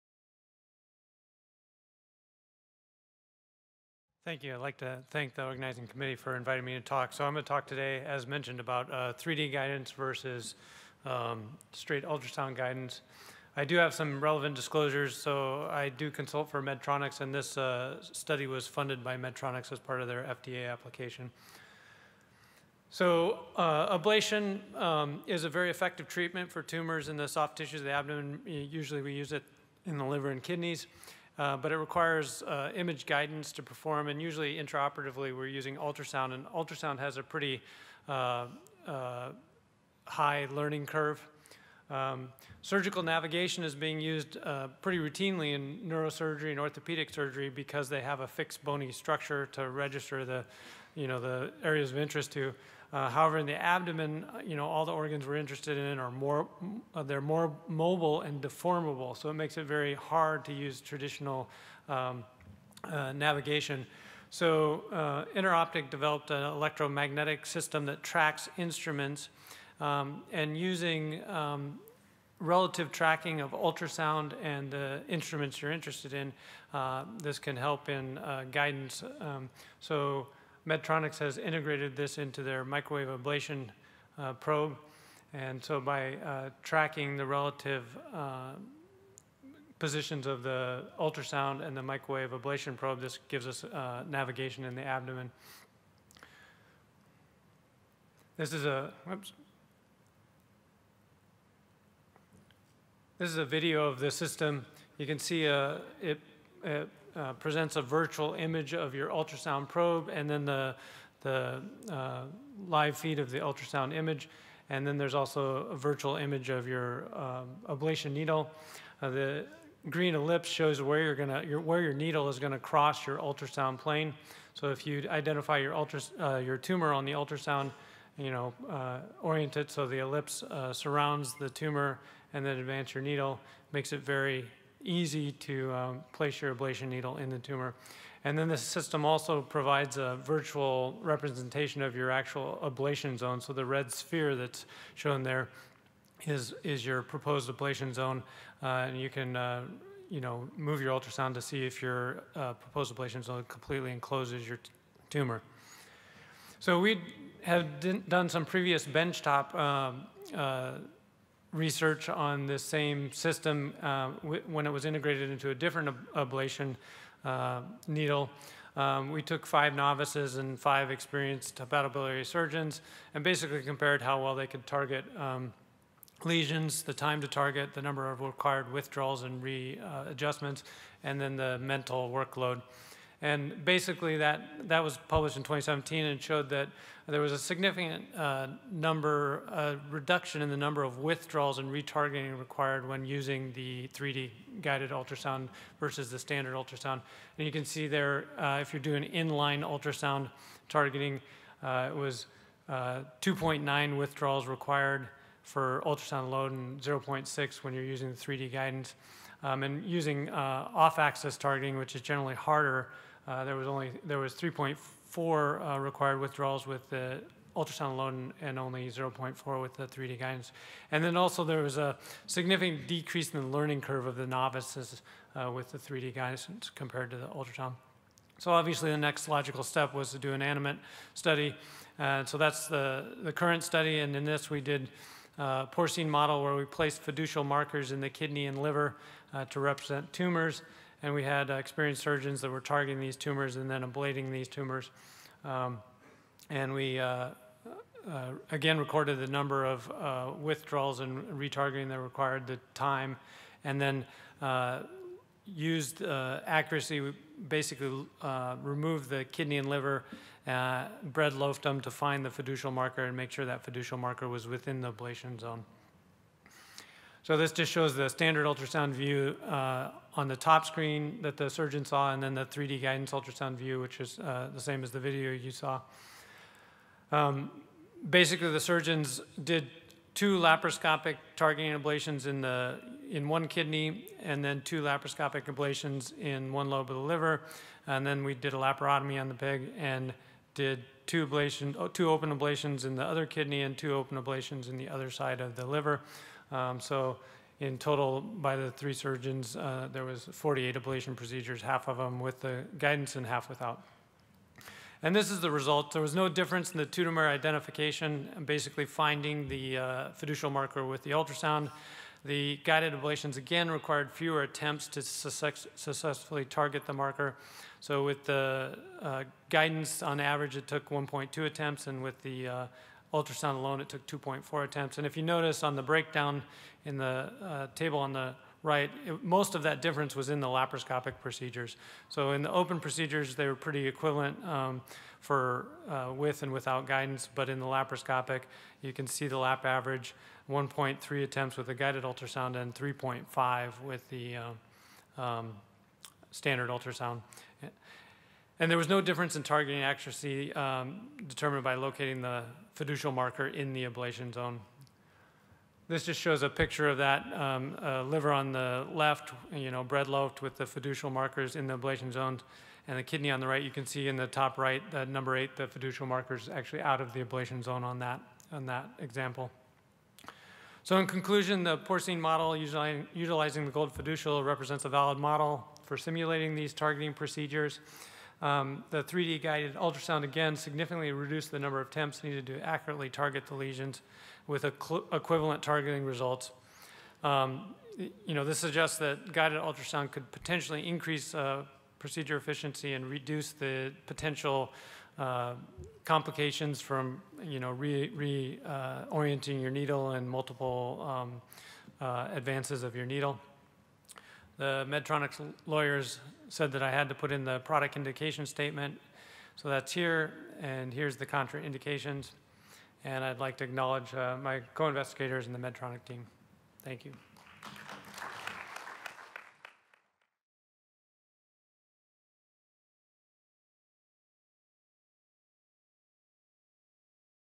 Tumor ablation using 3-D electromagnetic guided ultrasound vs standard ultrasound in a porcine model
This talk was presented at the 2018 SAGES Meeting/16th World Congress of Endoscopic Surgery by C Hammill during the ET: Emerging Technology Session on April 12 2018